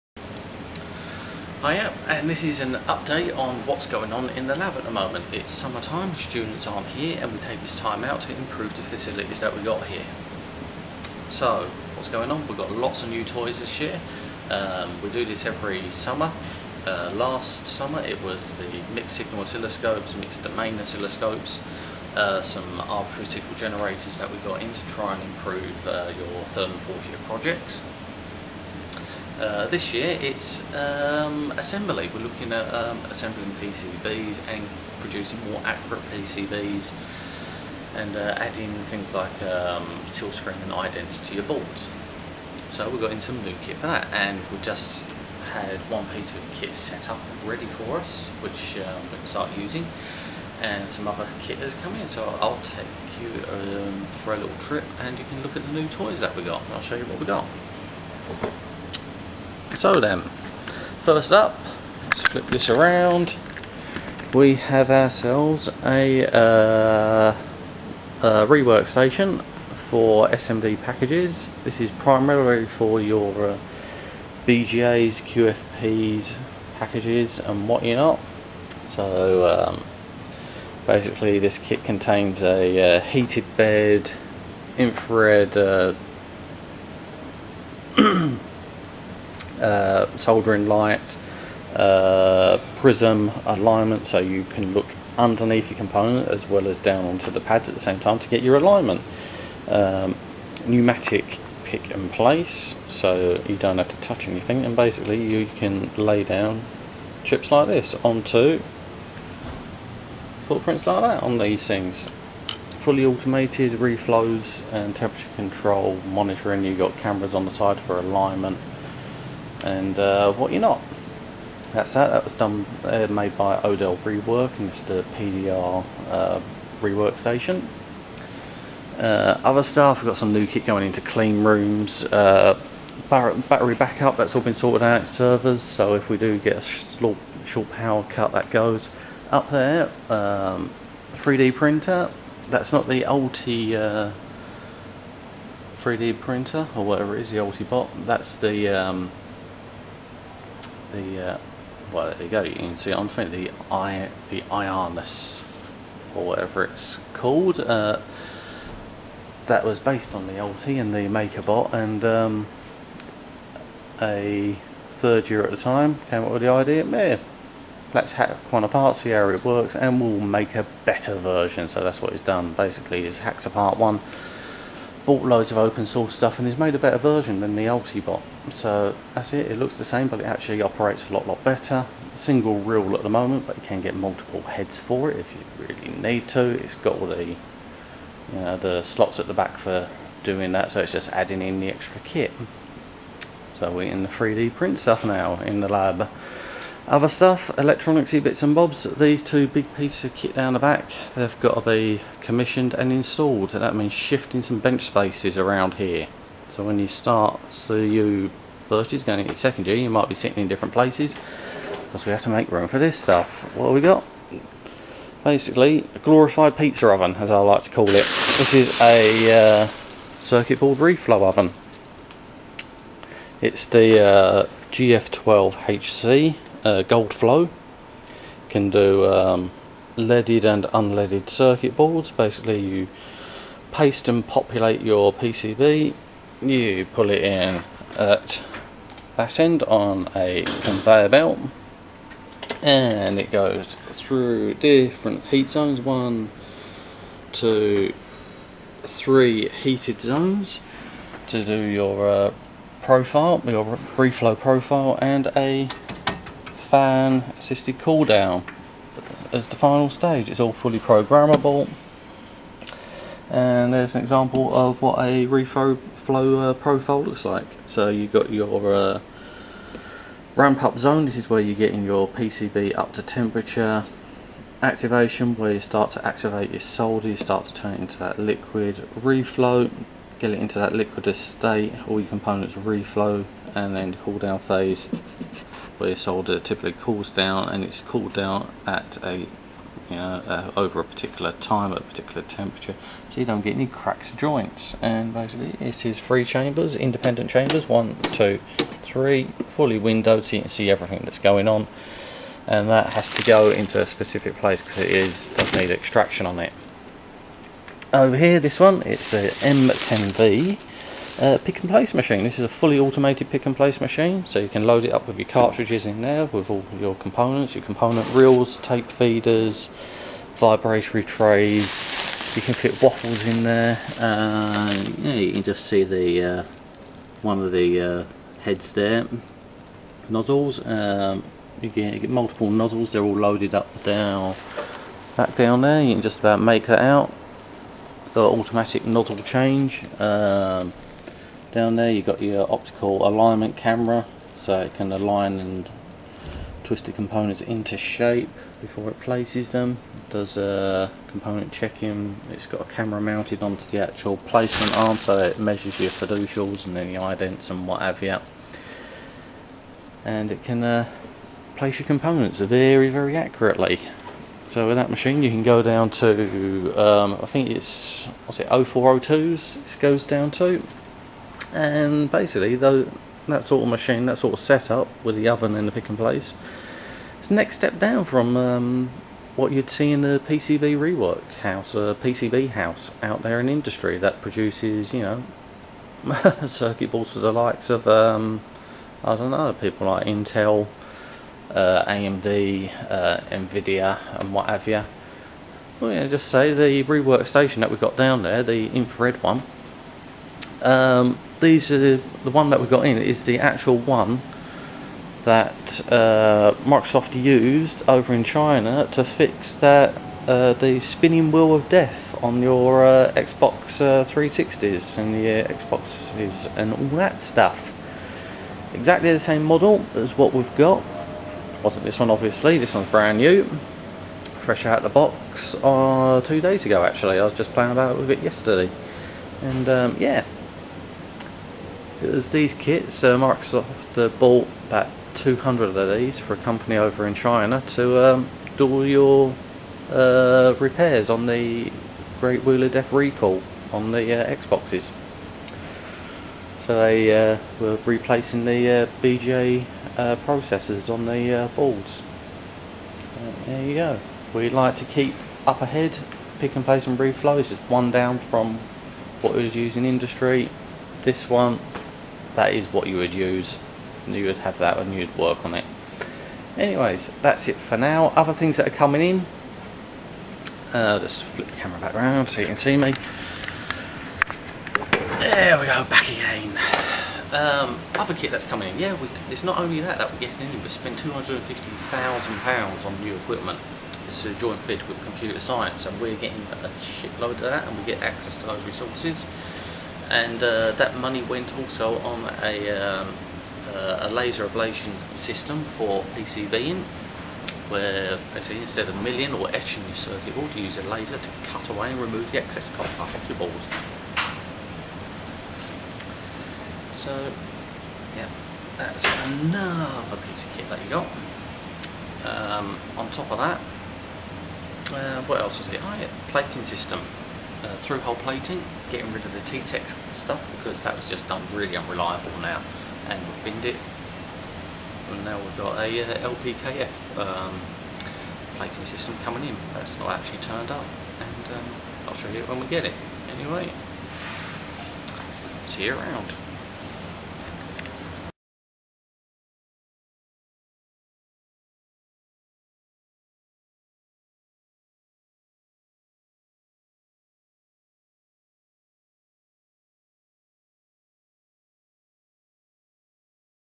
Whats happening in the UCL EE Teaching Lab for next term 2014
New toys are coming to the teaching lab at UCL Electronic Engineering for 2014, cheak out the new kit.

This is only a small sample of whats new to the lab for 2014 there is a whole heap of other stuff.